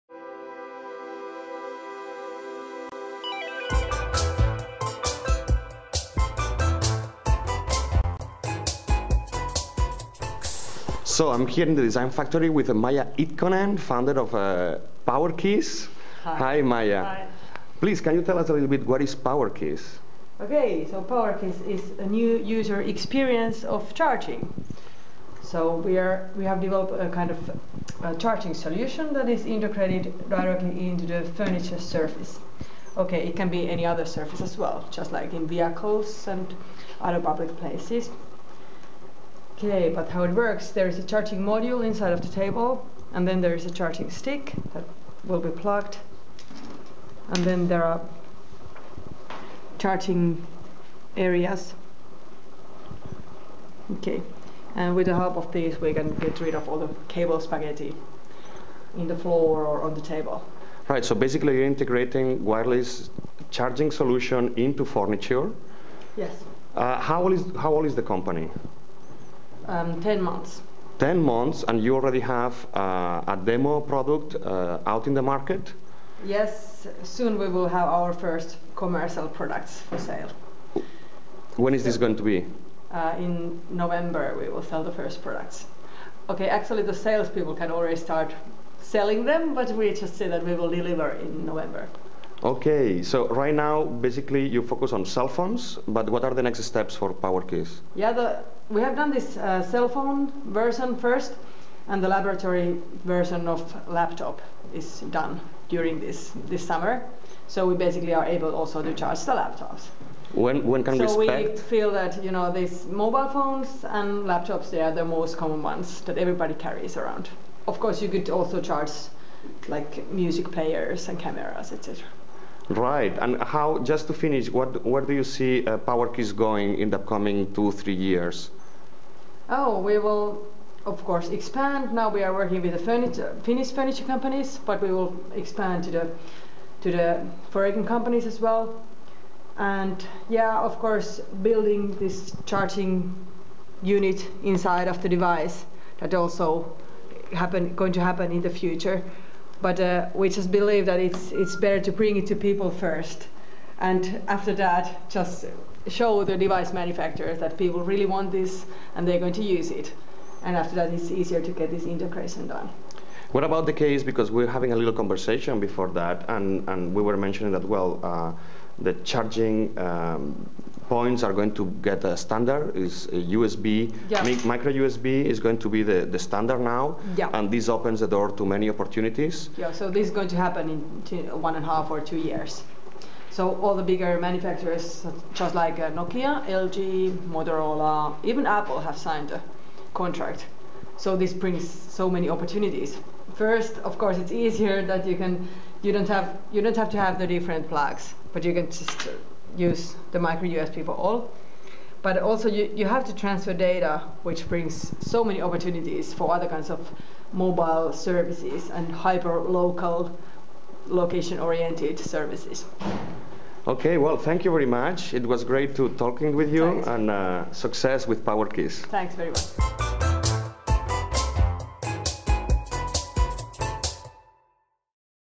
Aalto's Design Factory Startups: PowerKiss (wireless charging solutions)
During my visit to Finland, I had the chance to visit the Design Factory from Aalto University ; a passion-based co-creation platform. There, students, researchers, and entrepreneurs work together to create design solutions taking a user-centered approach.

One of the startups hosted at the Design Factory is PowerKiss, a company that uses induction technology integrated into forniture to charge mobile devices like cellphones and laptops. As Maija Itkonen (founder) explains in this video, PowerKiss is already taking orders for their products and commercialization is expected to start in two months time.

I personally love the concept; imagine being at home or at a cafe and by simply positioning your cellphone or laptop on the desk, it gets charged. Great! Moreover, with the adoption of micro-usb as standard charging plug in cellphones, the possibilites expand beyond charging, including transmission of data. In this case, hyper-local data; a single table can transmit different data depending in which part of the surface you are positioning your device.

It's time to get rid off the spaghetti cable!